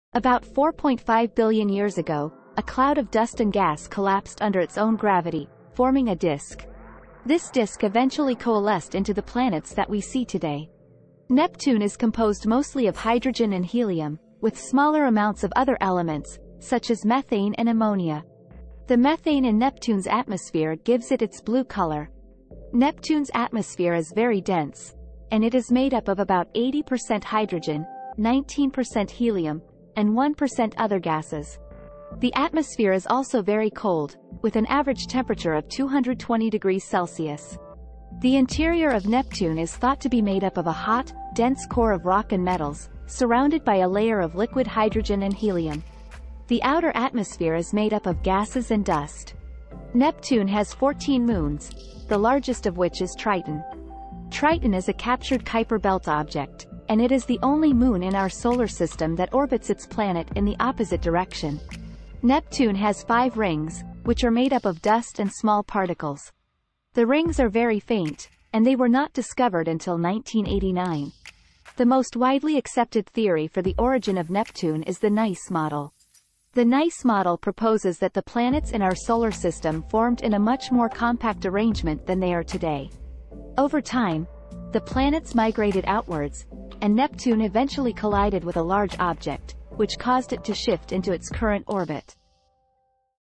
The Origin of Neptune
Neptune has five rings, which are made up of dust and small particles. The rings are very faint, and they were not discovered until 1989.